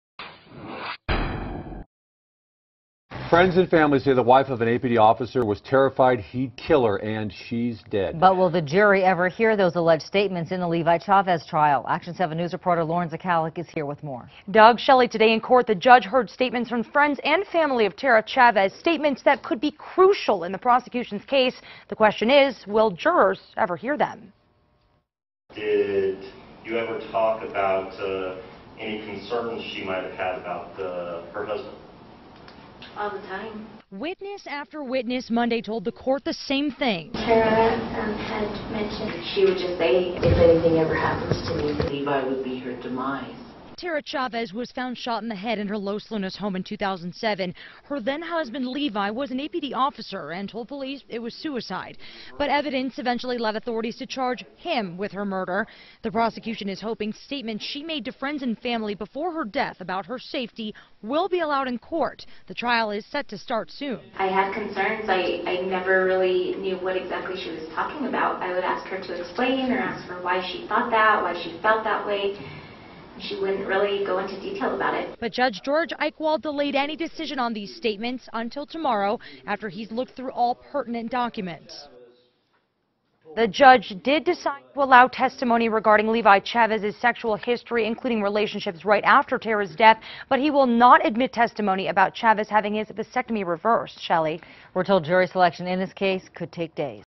NM officer's murder trial begins
Friends and family say the wife of an APD officer was terrified he'd kill her.  And she's dead.  But will the jury ever hear those alleged statements in the Levi Chavez trial?